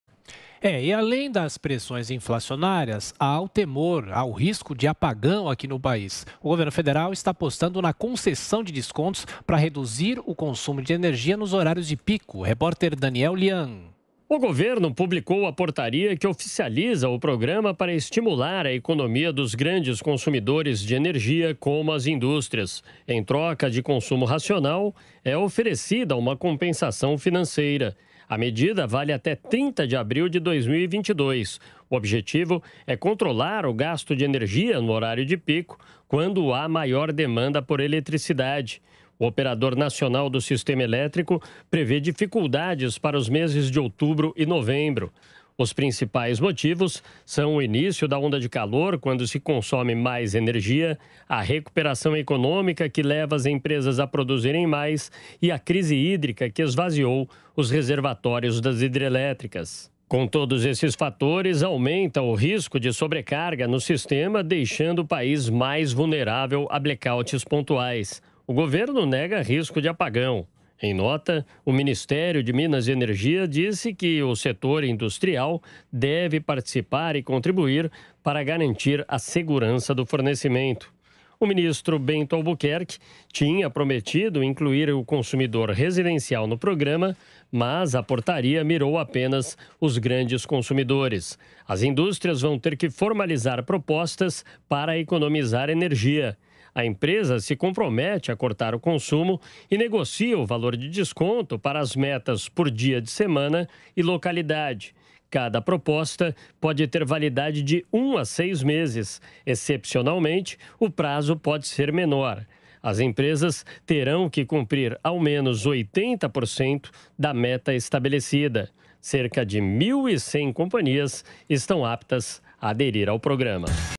Governo lança programa de incentivo a redução de consumo de energia na indústria
O governo federal publicou a portaria que oficializa o programa para estimular a economia dos grandes consumidores de energia, como as indústrias. Em troca de consumo racional, é oferecida uma compensação financeira. A medida vale até 30 de abril de 2022. O objetivo é controlar o gasto de energia no horário de pico, quando há maior demanda por eletricidade.